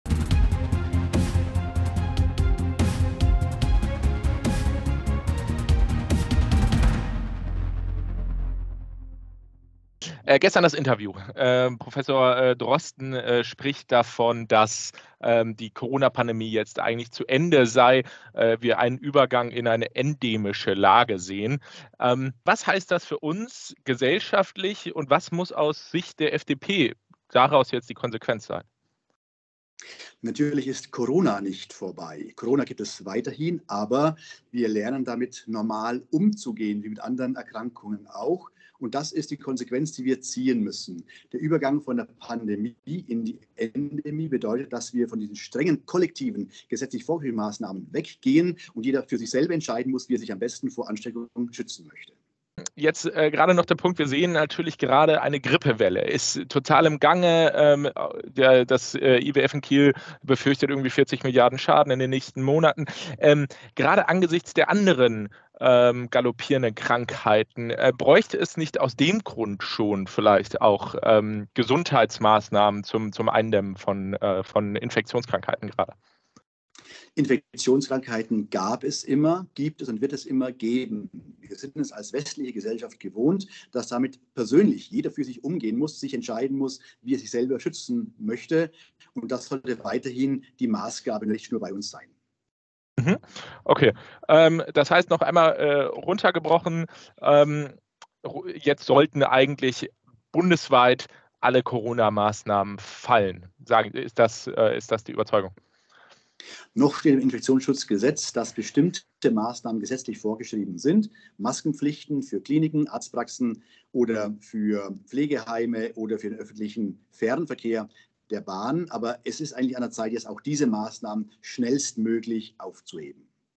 FDP-Politiker Stephan Thomae fordert: Corona-Maßnahmen "schnellstmöglich aufheben" | EXKLUSIV
Nach dem Ausrufen des Pandemie-Endes durch Christian Drosten geht die FDP aufs Ganze. Es solle keine gesetzlich vorgeschriebenen Einschränkungen mehr geben, fordert Stephan Thomae.

Eure ProSieben-newstime